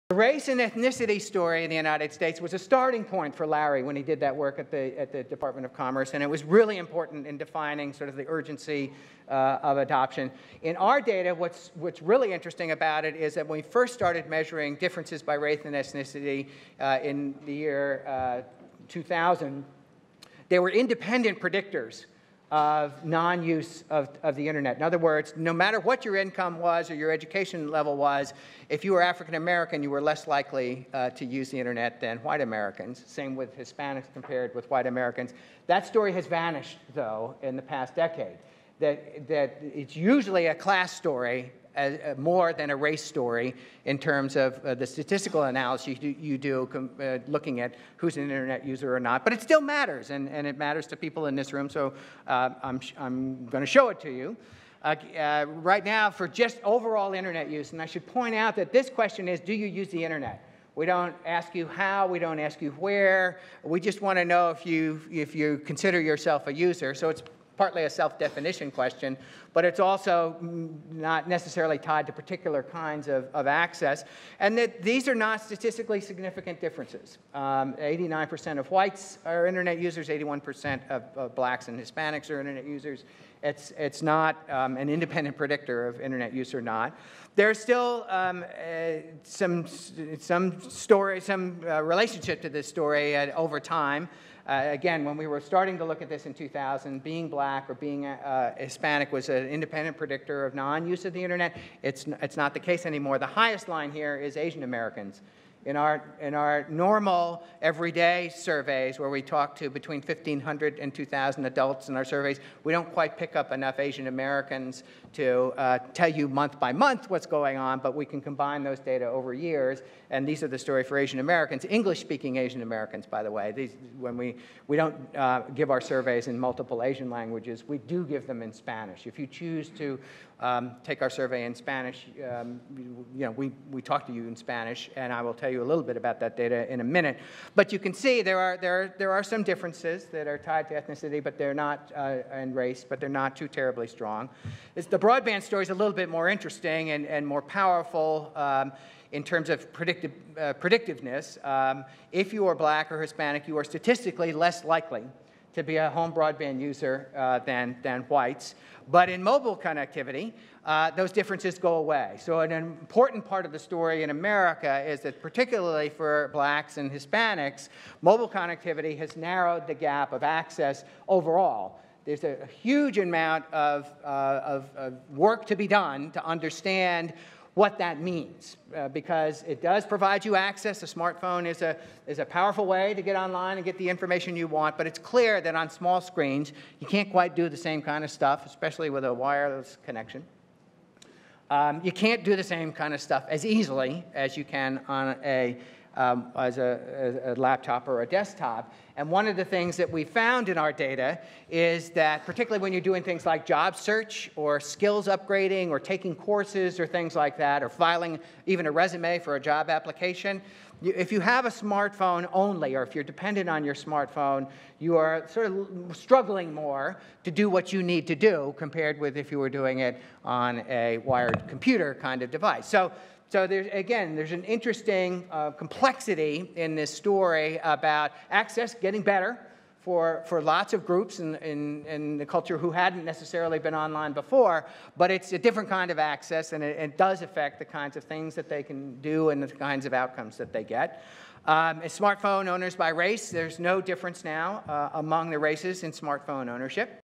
Lee Rainie talks about race, ethnicity, and Internet
Lee Rainie, Director of Internet, Science, and Technology at the Pew Research Center, explains the differences in Internet usage and what technology is used to consume the Internet between different races and ethnicities.